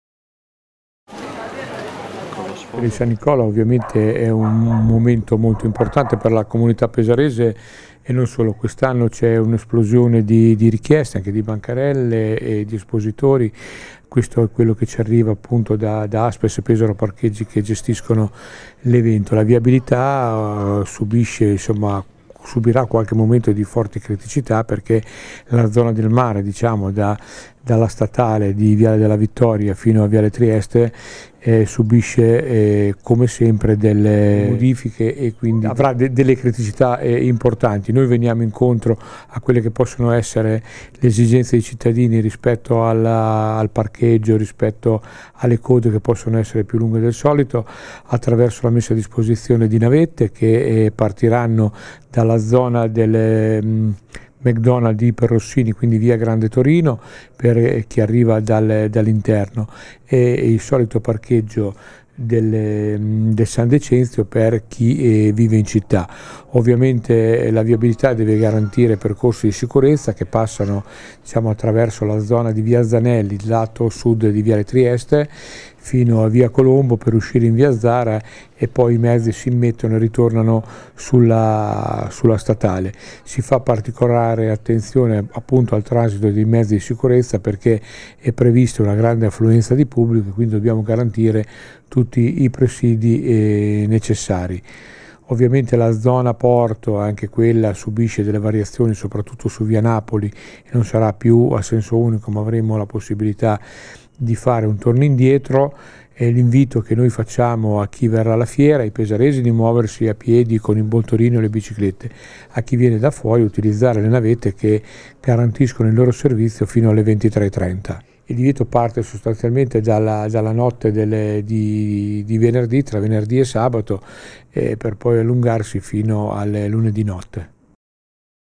Fiera di San Nicola: modifiche alla viabilità
Da oggi a domenica torna a Pesaro la Fiera di San Nicola che comporta diverse modifiche alla viabilità: ecco quali saranno le zone chiuse al traffico e i servizi di trasporto per i visitatori.